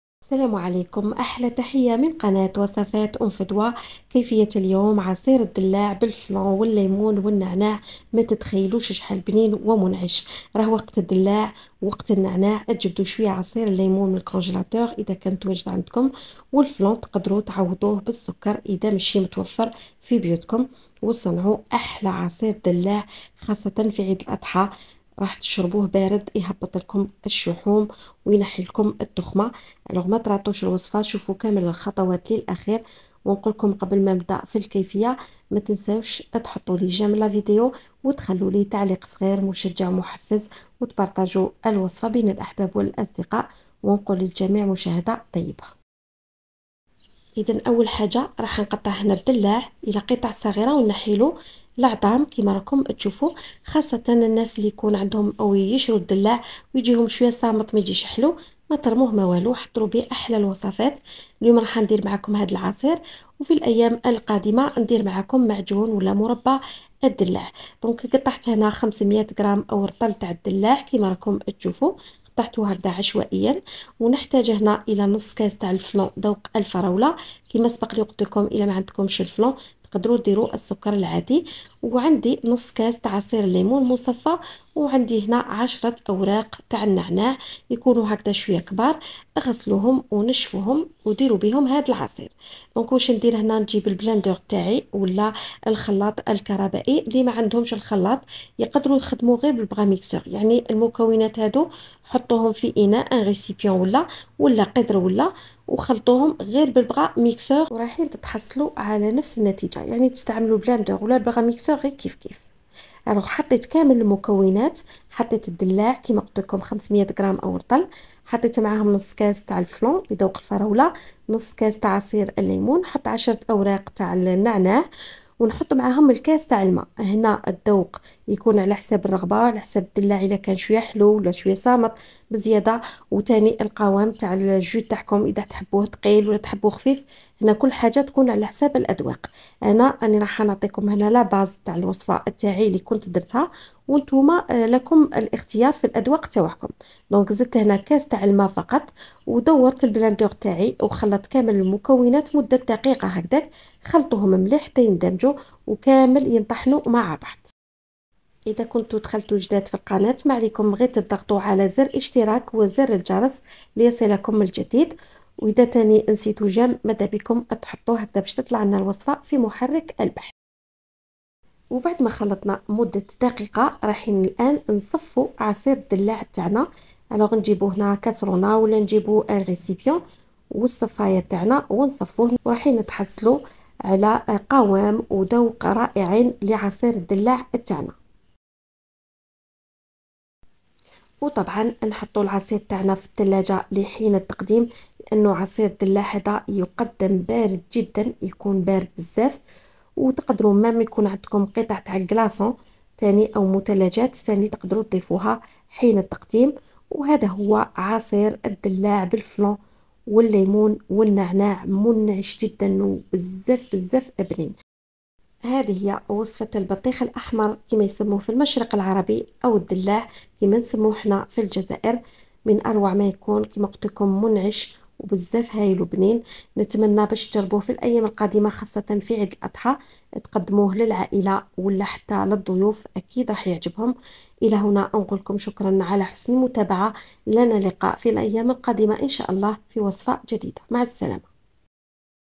عصير الدلاع و الليمون🍉🍋اوعصير البطيخ الاحمر قمة الانتعاش/jus de pastèque
المقادير:
500غ دلاع(بطيخ احمر)منزوع النواة-نصف كاس فلان بذوق الفراولة
نصف كاس عصير الليمون-10 اوراق نعناع -كاس ماء